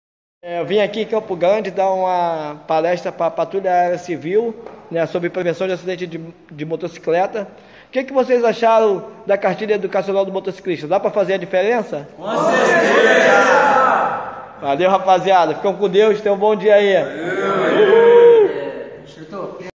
Alfredo Cartilha Palestrando para Patrulha Aérea Civil
Na base Centro Esportivo Miécimo da Silva, a Convite do Comandante Nilton Miranda coronel da Patrulha Aérea Civil, e André Lap.